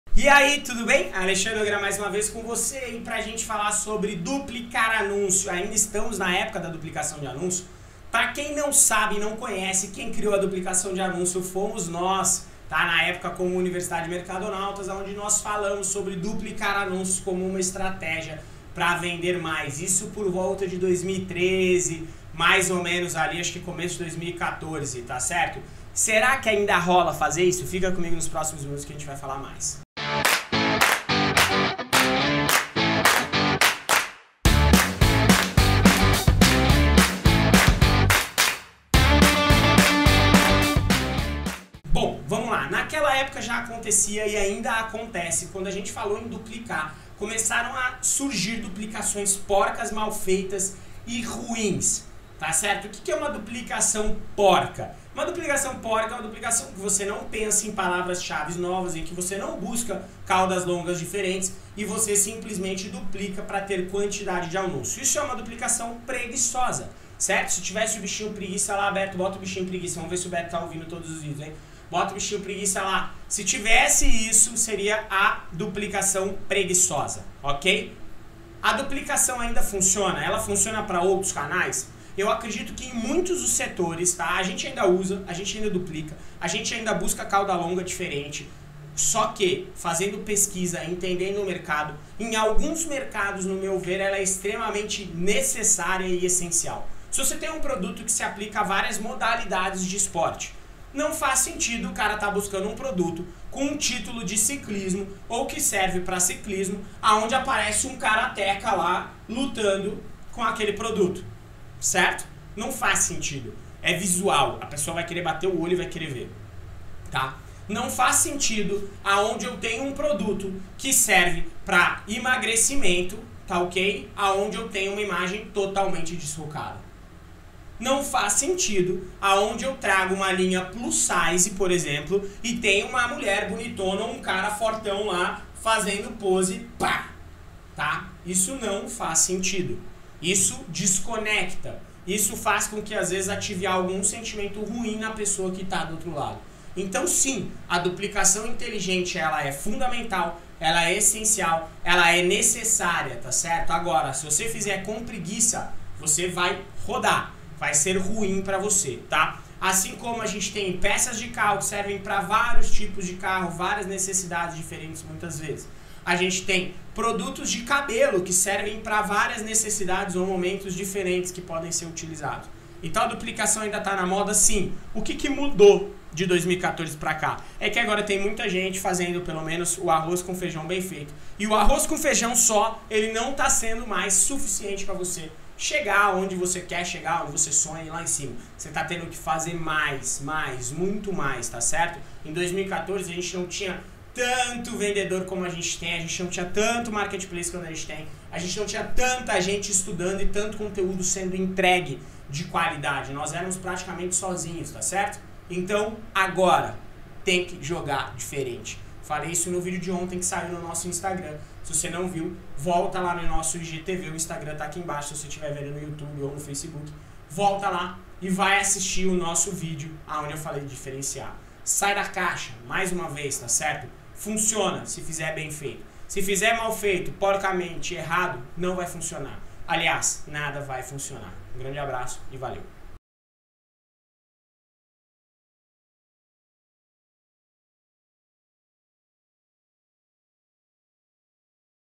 Duplicar anúncios no Mercado Livre e Marketplaces compensa?
Ainda estamos na época de duplicar anúncios?

Lá atrás quando ainda éramos Universidade Mercadonautas idealizamos esse processo, mas depois desses anos será que ainda vale a pena a duplicação?
Acaba por ser uma facilidade para quem precisa aumentar o número de anúncios, só que desde lá atrás surgiam muitos anúncios porcos, mal feitos e sem direcionamento que por vezes acabavam funcionando pois o mercado de Marketplaces ainda era pouco explorado!

Mas e atualmente com um mercado cada vez mais competitivo, e tanta gente estudando como vender nos Marketplaces será que ainda é possível usar a duplicação para aumentar o leque de anúncios?

Fique ligado, se quer ver o vídeo de ontem (13/02) só ir lá no instagram @alevnogueiraa ou clicar aqui

Tamo junto e grande abraço.